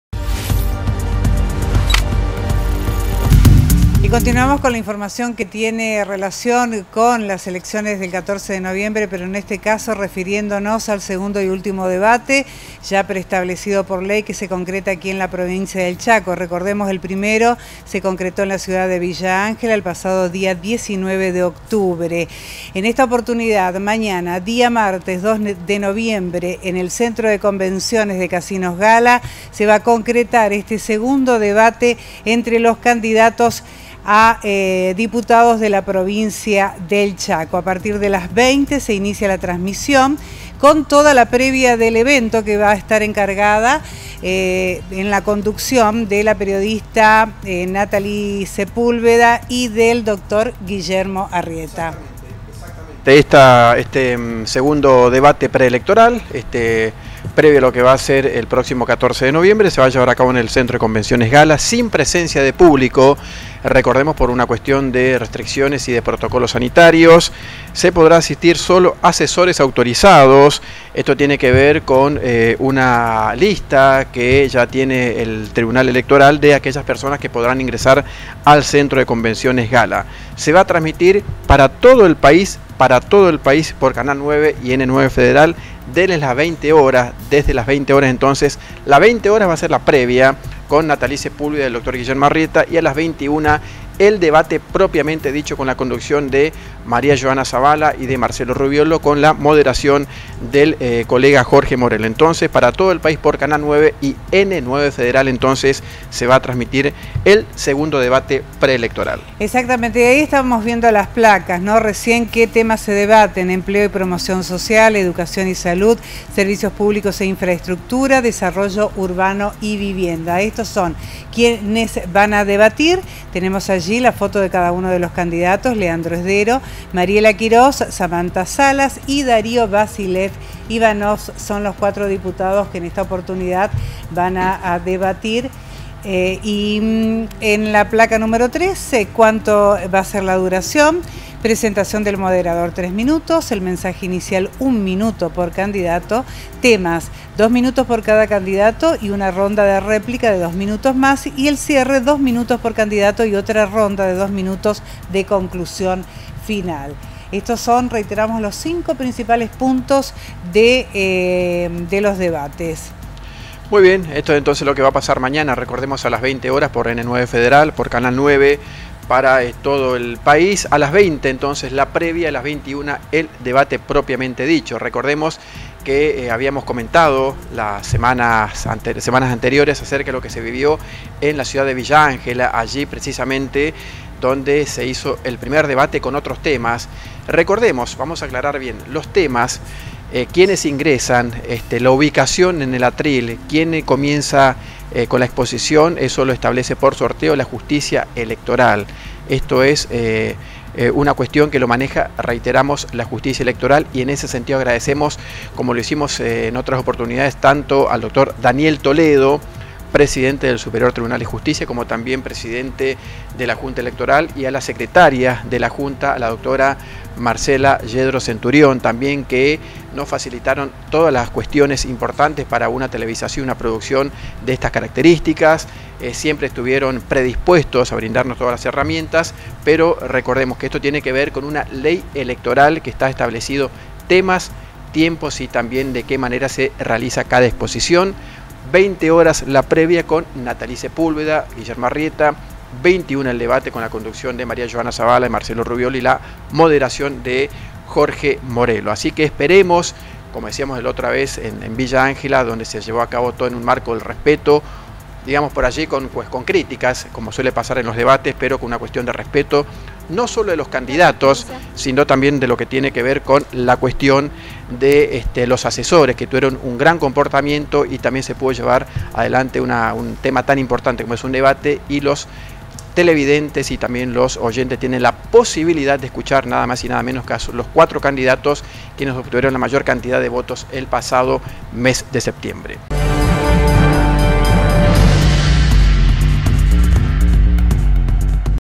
N9 FEDERAL - DECISIÓN 2021 - LLEGA “EL DEBATE” SEGUNDO ENCUENTRO DE CANDIDATOS
A LAS 20:00 HORAS INICIA LA TRANSMISIÓN CON LA PREVIA DEL EVENTO
A LAS 21:00 HORAS COMIENZA EL DEBATE
DEBATIRÁN SOBRE: EMPLEO Y PROMOCIÓN SOCIAL; EDUCACIÓN Y SALUD
DEBATIRÁN SOBRE: SERVICIOS PÚBLICOS E INFRAESTRUCTURA / DESARROLLO URBANO Y VIVIENDA